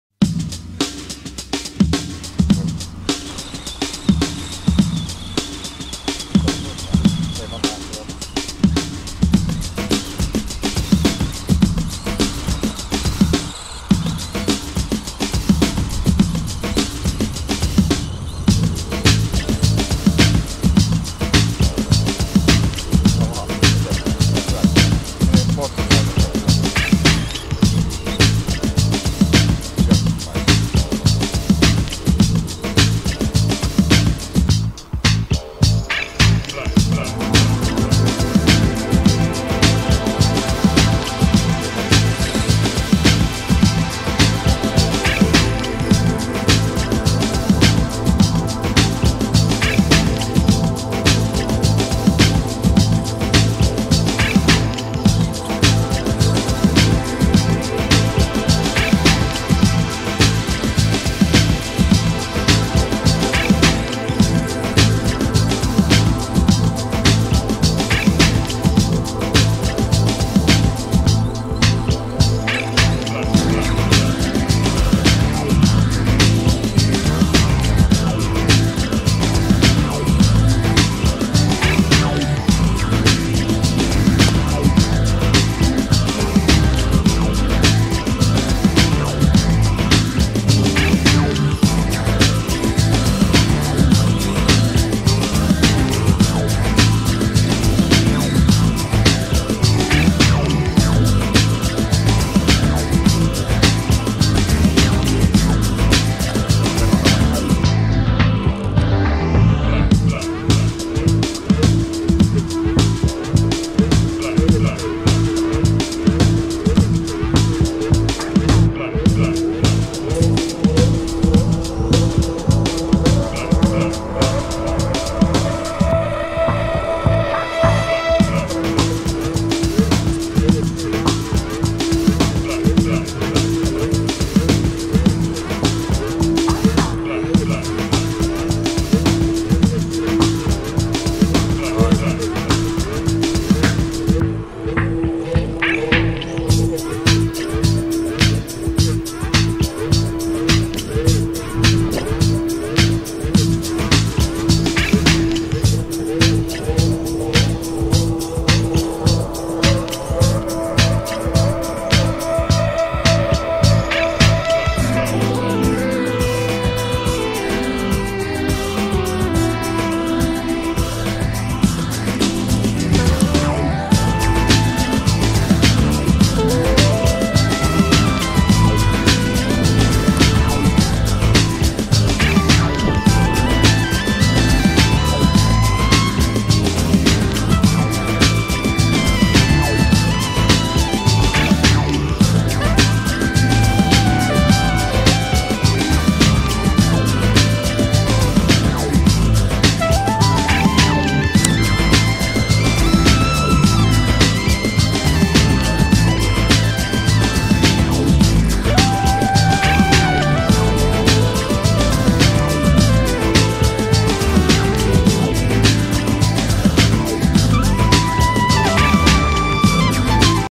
Driving with RC-cars (Bullet ST,Mini recons)
Watch when RCs take over the asphalt and gravel!

Cars we used in this video: 2 HPI Mini recons and HPI Bullet ST Flux

Where we bought our RCs:Hobbylinna.fi